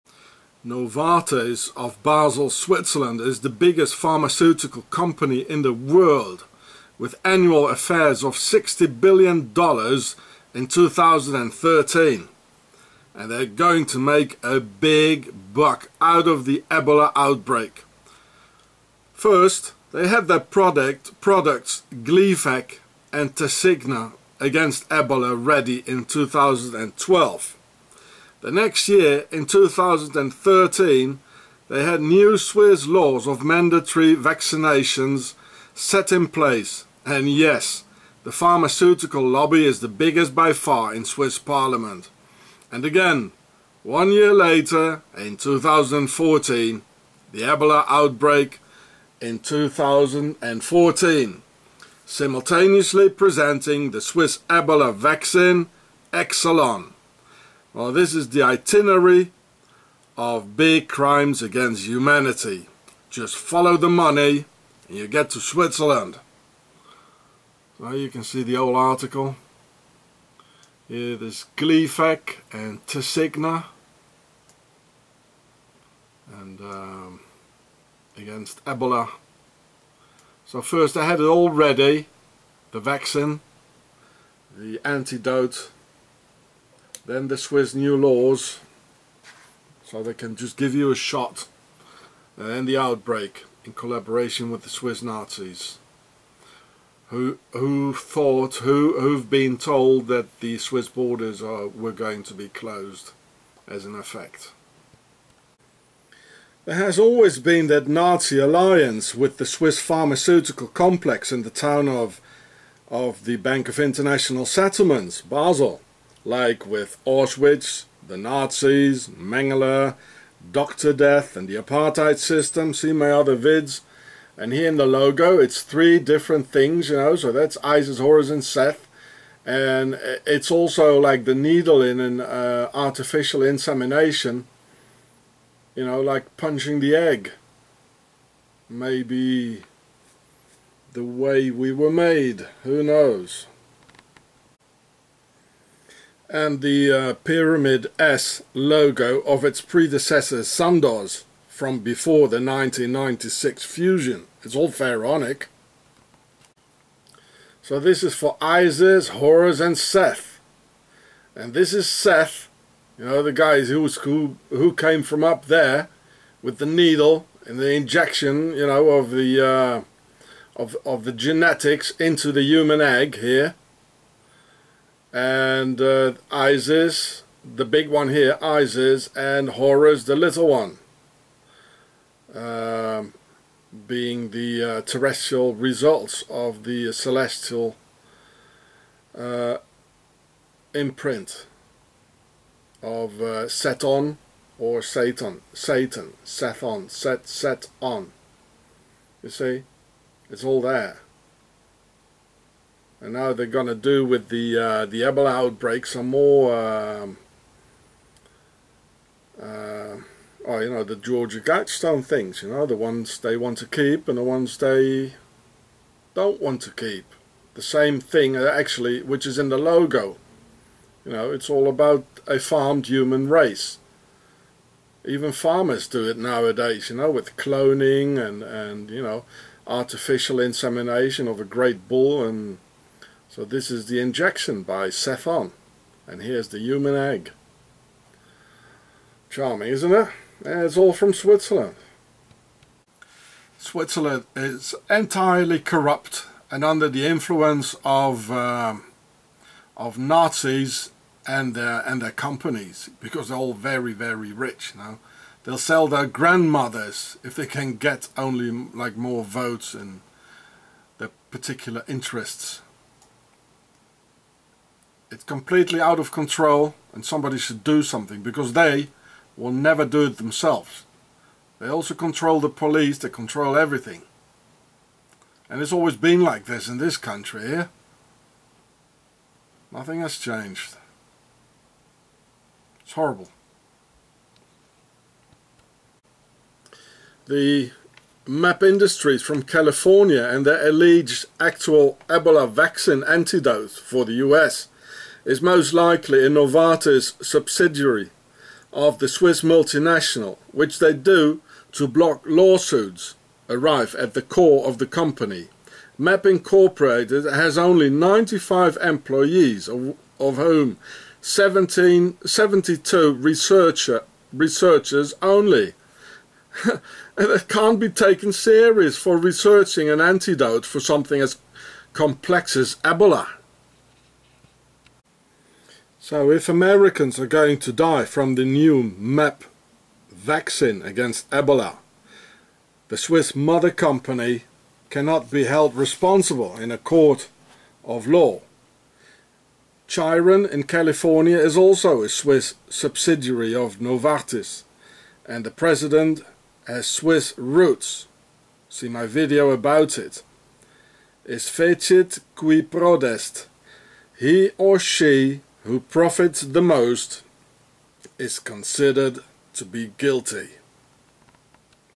Swiss Novartis Ebola Vaccine 2012: Itinerary of Crime against Humanity by Switzerland
THE ITINERARY OF SWISS CRIME: 2012 EBOLA ANTIDOTE READY IN SWITZERLAND, 2013 SWISS MANDATORY VACCINATION LAWS AND 2014 THE ACTUAL EBOLA OUTBREAK; so the Swissies have it all set in place now - the vaccine, the new pandemic laws and now the real pandemic. So soon they can force to inject us whatever they want and simultaneously make a hell of a lot of money with Gleevec and Tasigna.